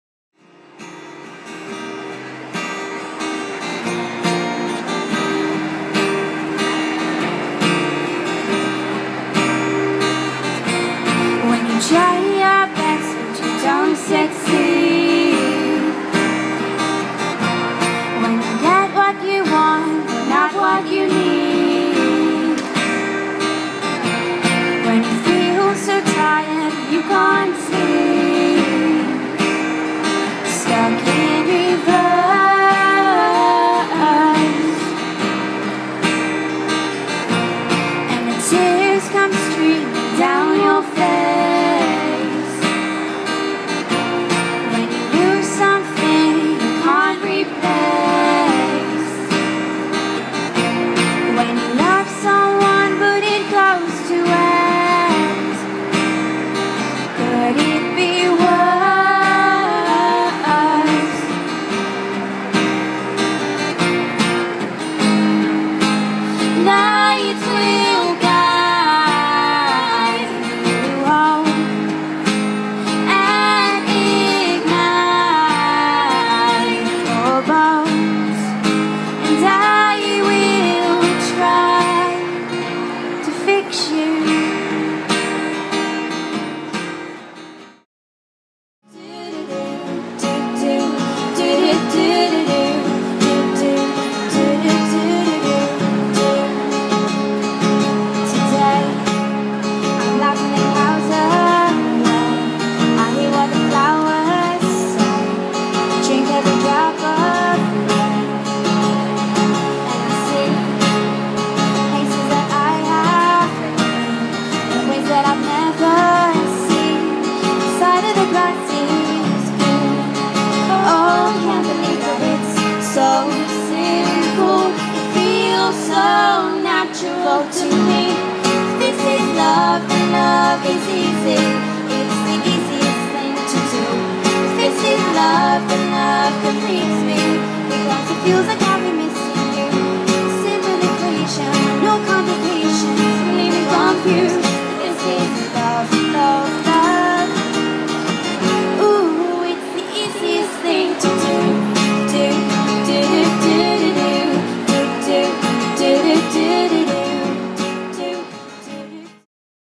Grace in Forum Live at the LG Arena
Grace performing in Forum Live at the LG Arena before The X Factor Live Tour 2013 on 10 February 2013